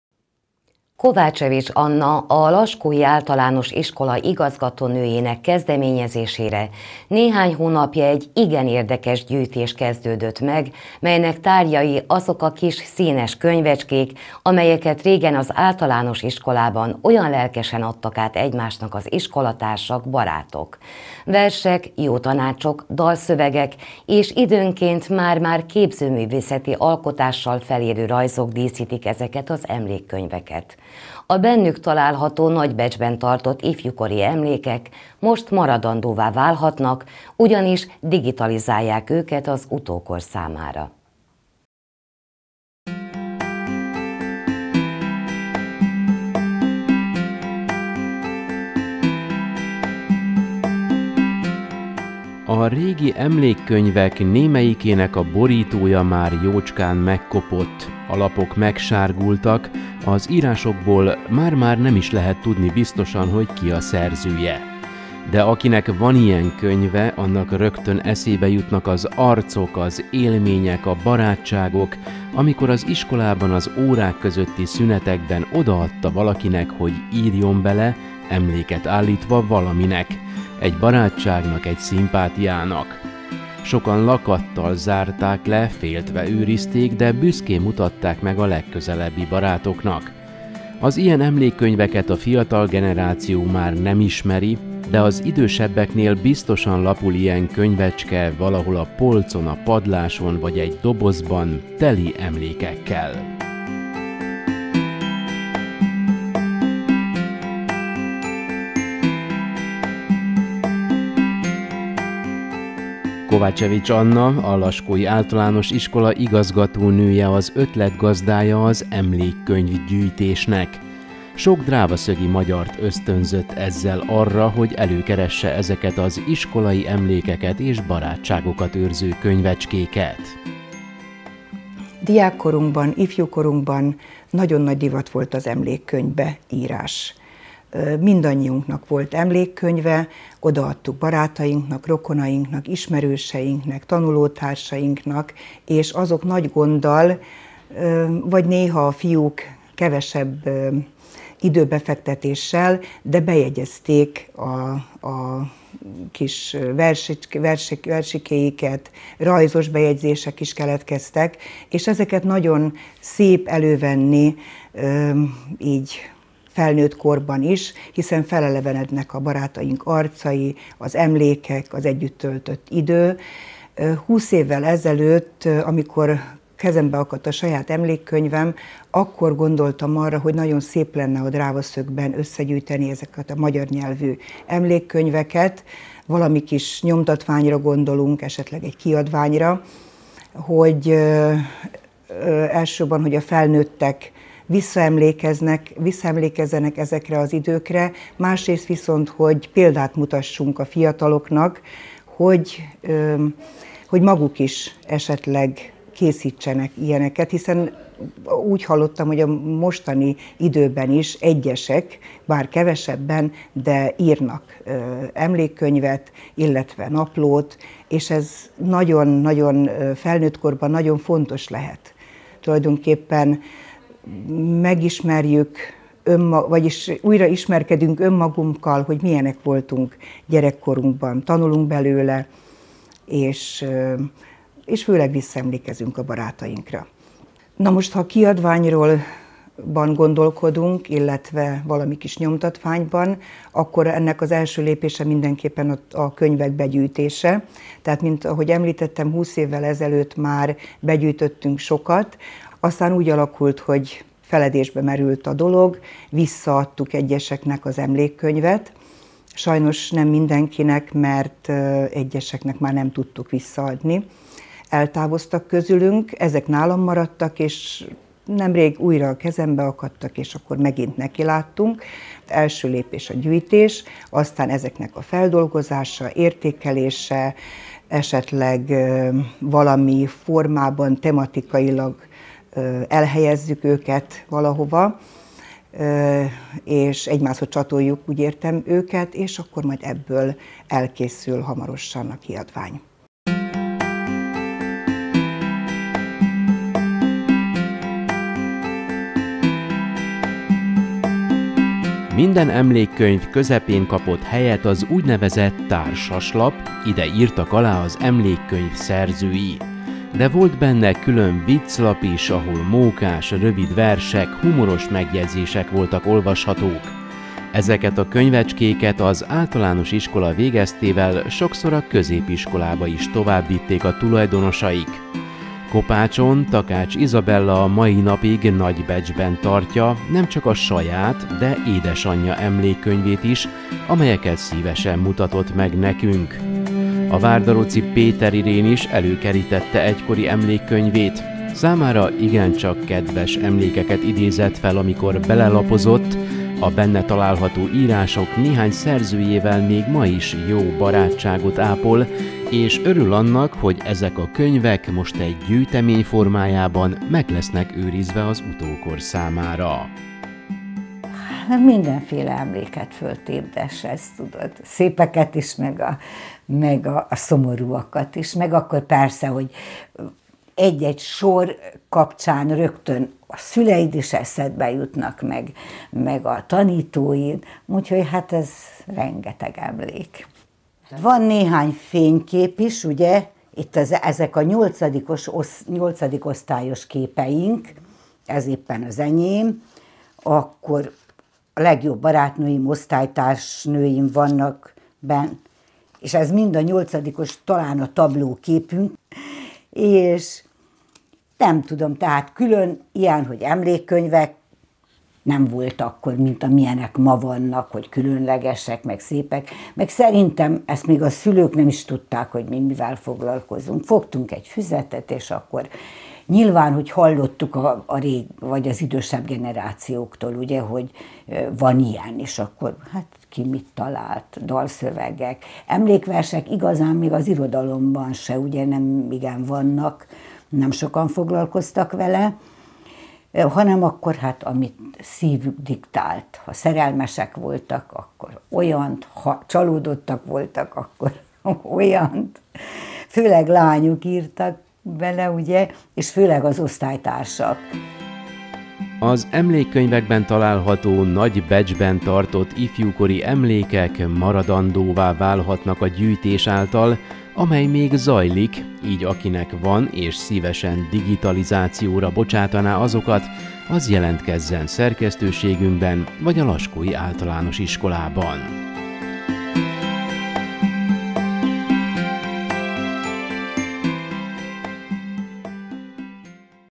DRÁVATÁJ - Összegyűjtik és digitalizálják az egykor népszerű emlékkönyveket
érdekes gyűjtés kezdődött meg, melynek tárgyai azok a kis, színes könyvecskék, amelyeket régen az általános iskolában olyan lelkesen adtak át egymásnak az iskolatársak, barátok. Versek, jó tanácsok, dalszövegek és időnként már-már képzőművészeti alkotással felérő rajzok díszítik ezeket az emlékkönyveket. A bennük található nagy becsben tartott ifjúkori emlékek most maradandóvá válhatnak, ugyanis digitalizálják őket az utókor számára.